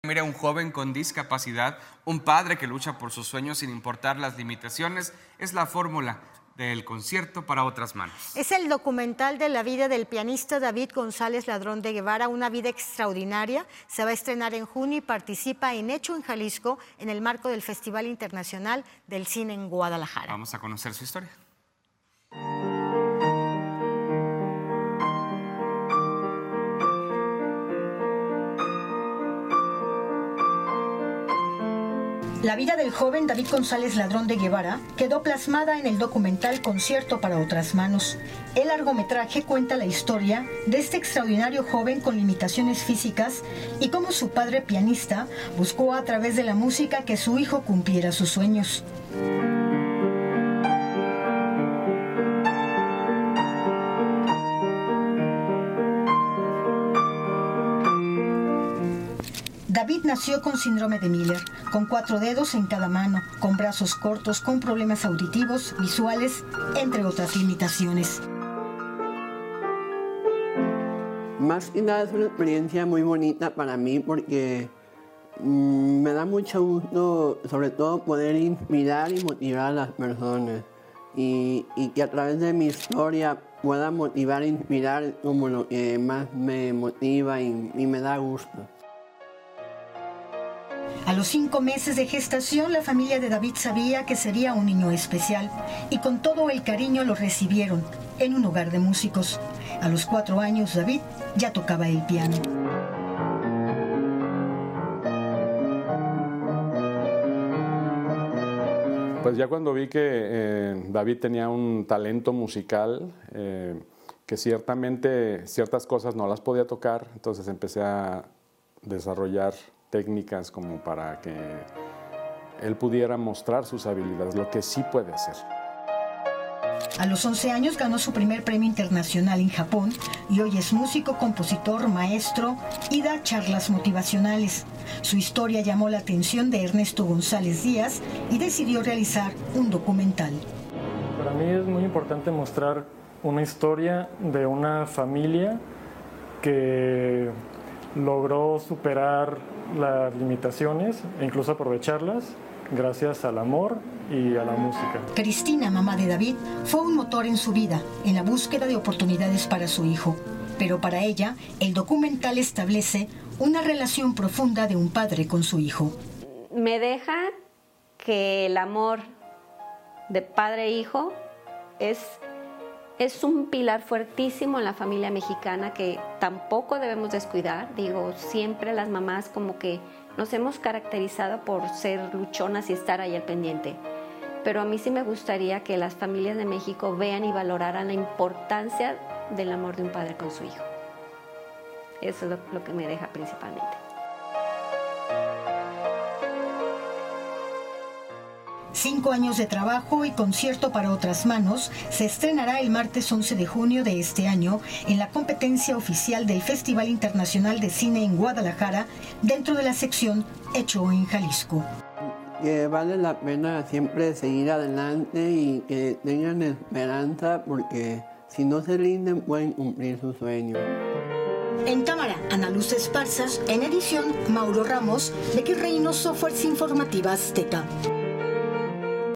Documental Concierto para Otras Manos
La vida del joven David González Ladrón de Guevara quedó plasmada en el documental “Concierto para Otras Manos”; el largometraje cuenta la historia de este extraordinario joven. La información con Becky Reynoso